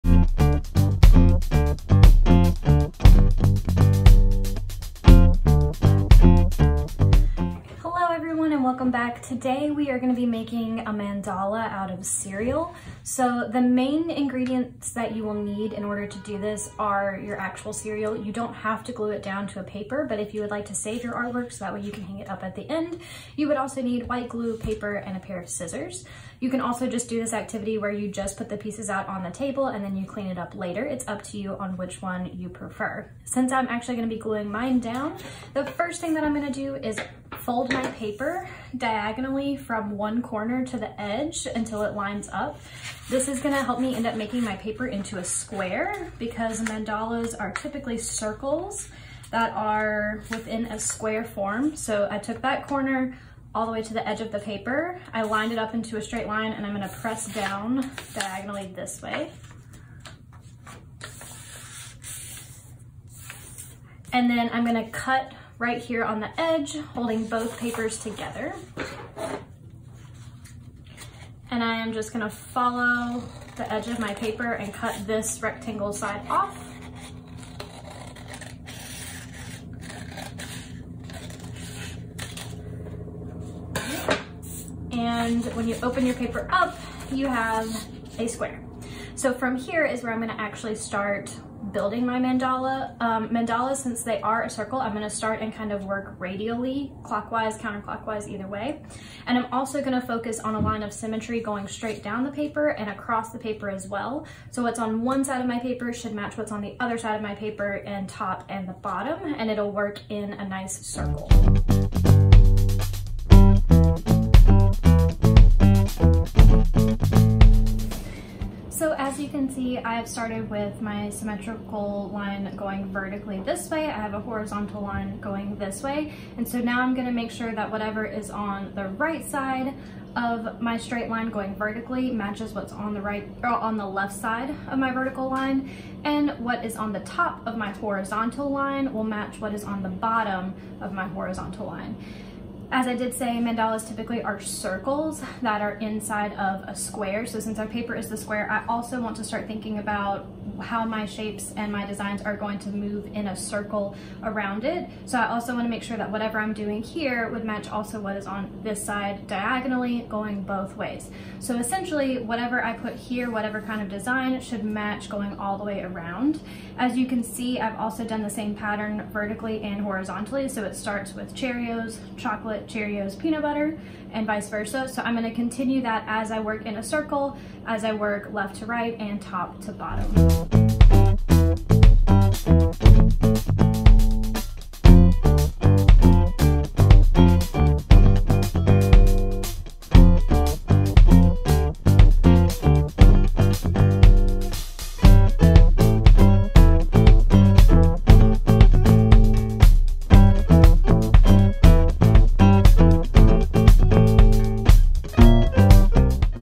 Cereal Mandalas
How to make a mandala out of simple supplies that you probably have at home! Mainly out of cereal but can be glued to paper to create a final art piece!